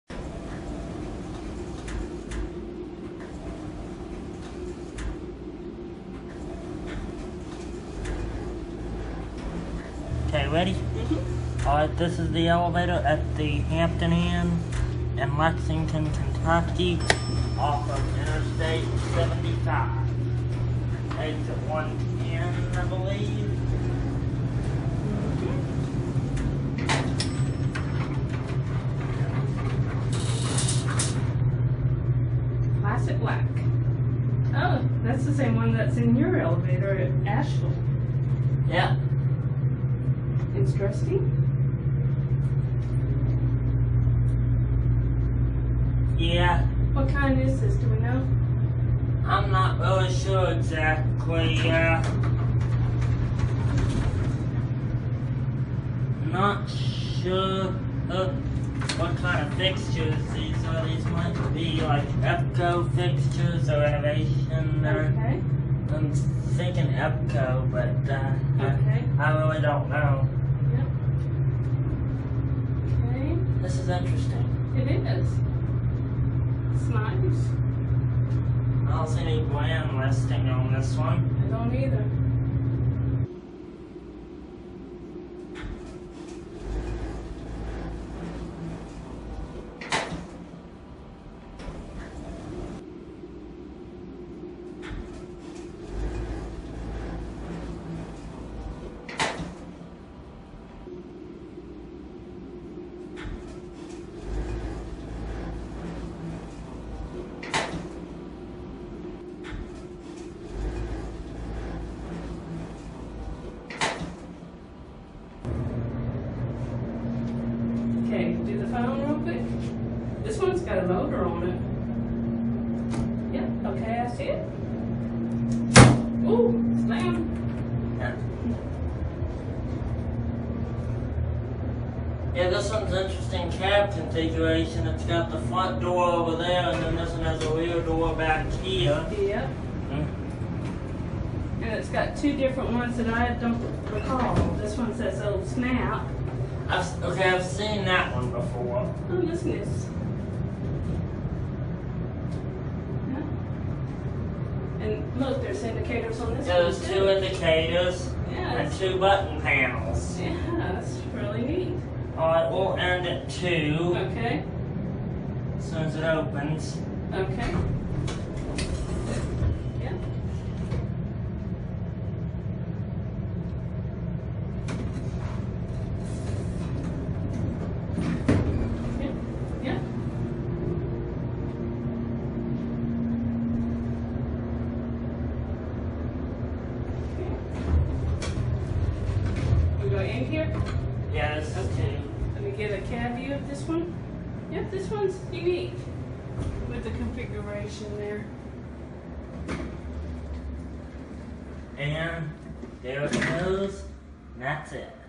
Elevator at the Hampton Inn in Lexington / Hamburg I-75 Kentucky
This was a nice Hampton Inn to stop at & explore. Not sure what brand of elevator. I think the buttons are epco or innovation though.  For more info on this particular Hampton, check out their website at